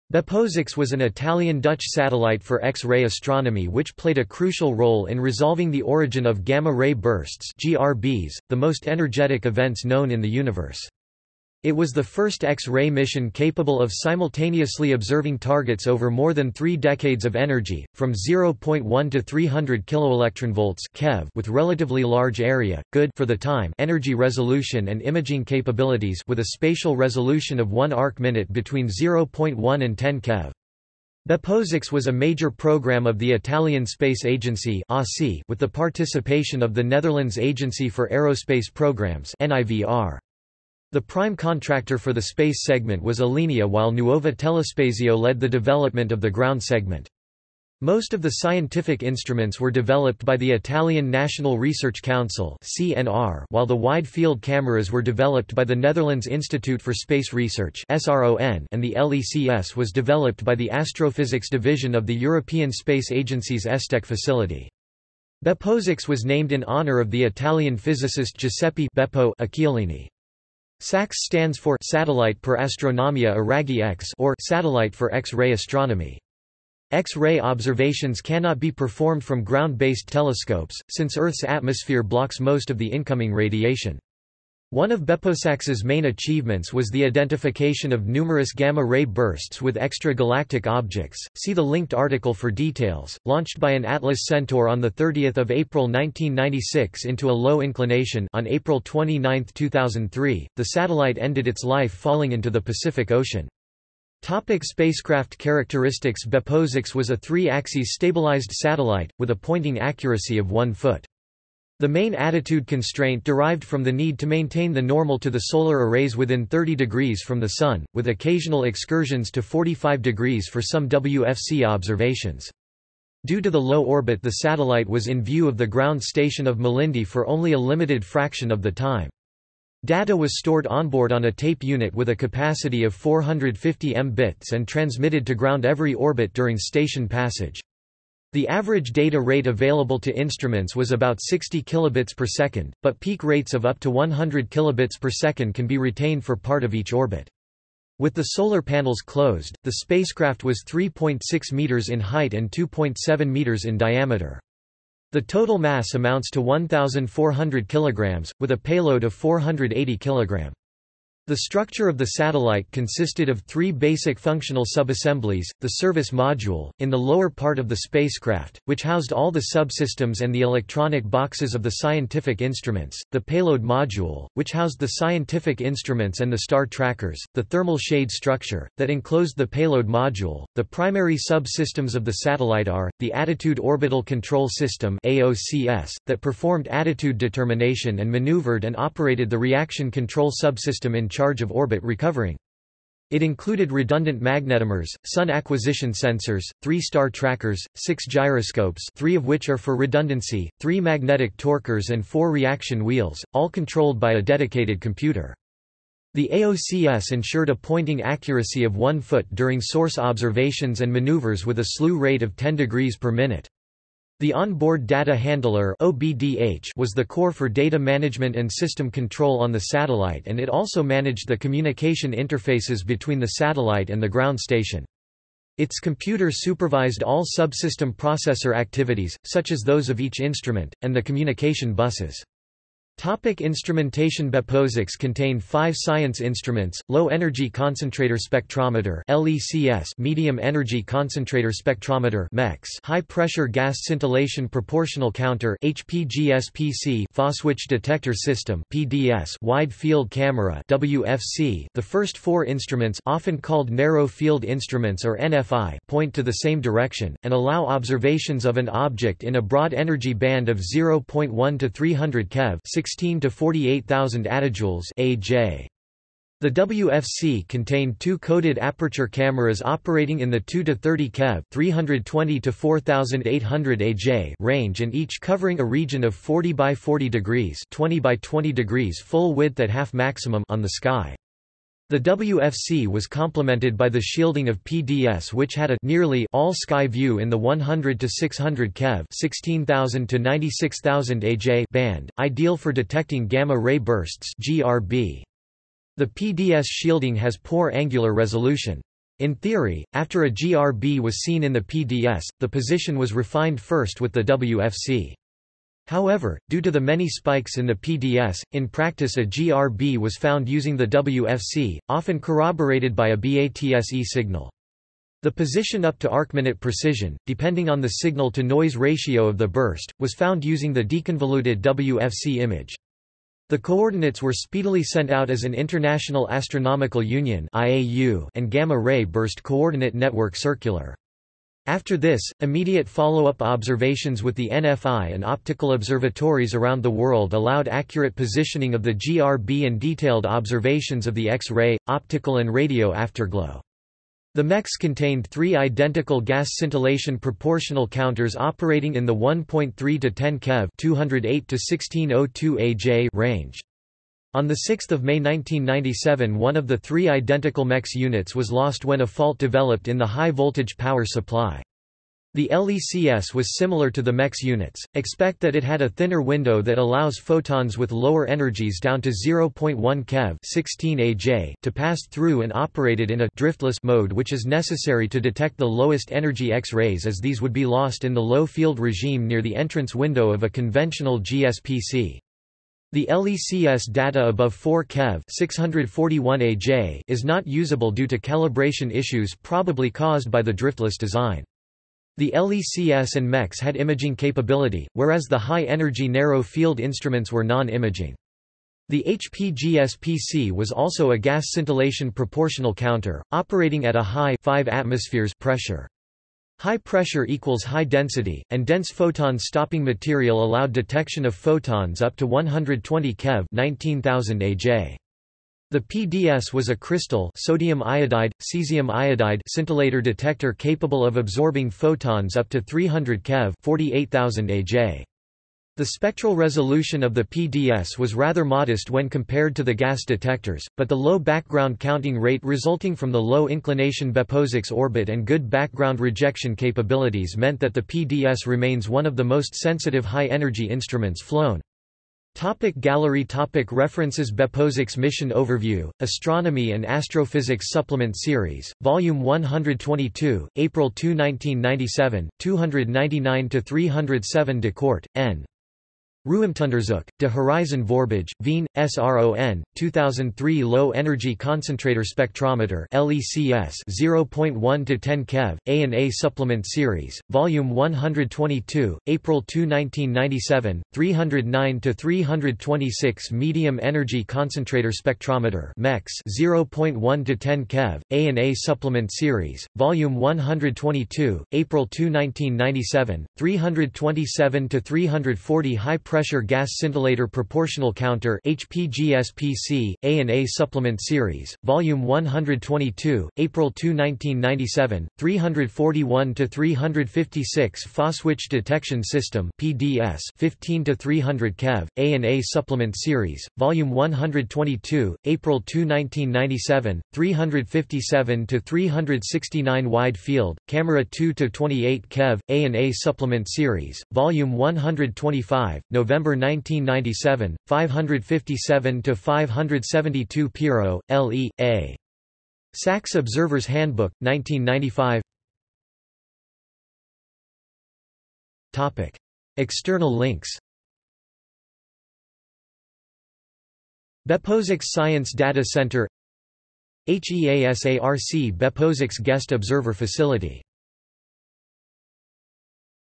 BeppoSAX | Wikipedia audio article
This is an audio version of the Wikipedia Article:
BeppoSAX

- Socrates

SUMMARY
BeppoSAX was an Italian–Dutch satellite for X-ray astronomy which played a crucial role in resolving the origin of gamma-ray bursts (GRBs), the most energetic events known in the universe. It was the first X-ray mission capable of simultaneously observing targets over more than 3 decades of energy, from 0.1 to 300 kiloelectronvolts (keV) with relatively large area, good (for the time) energy resolution and imaging capabilities (with a spatial resolution of 1 arc minute between 0.1 and 10 keV). BeppoSAX was a major programme of the Italian Space Agency (ASI) with the participation of the Netherlands Agency for Aerospace Programmes (NIVR). The prime contractor for the space segment was Alenia while Nuova Telespazio led the development of the ground segment. Most of the scientific instruments were developed by the Italian National Research Council (CNR) while the Wide Field Cameras were developed by the Netherlands Institute for Space Research (SRON) and the LECS was developed by the astrophysics division of the European Space Agency's ESTEC facility.
BeppoSAX was named in honour of the Italian physicist Giuseppe "Beppo" Occhialini. SAX stands for "Satellite per Astronomia a raggi X" or "Satellite for X-ray Astronomy".
X-ray observations cannot be performed from ground-based telescopes, since Earth's atmosphere blocks most of the incoming radiation.
One of BeppoSAX's main achievements was the identification of numerous gamma ray bursts with extra-galactic objects. (See the linked article for details.)
Launched by an Atlas-Centaur on 30 April 1996 into a low inclination (4 degree) low-Earth orbit, the expected operating life of two years was extended to April 30, 2002 due to high scientific interest in the mission and the continued good technical status. After this date, the orbit started to decay rapidly and various subsystems were starting to fail making it no longer worthwhile to conduct scientific observations.
On April 29, 2003, the satellite ended its life falling into the Pacific Ocean.